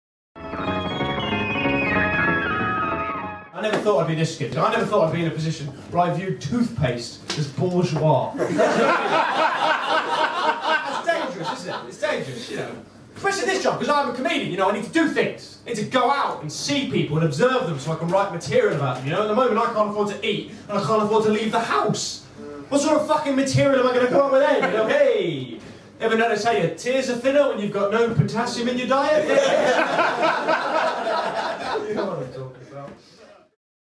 Funny Side Up! Presents...Sean McLoughlin
Sean McLoughlin on the lighter side of poverty...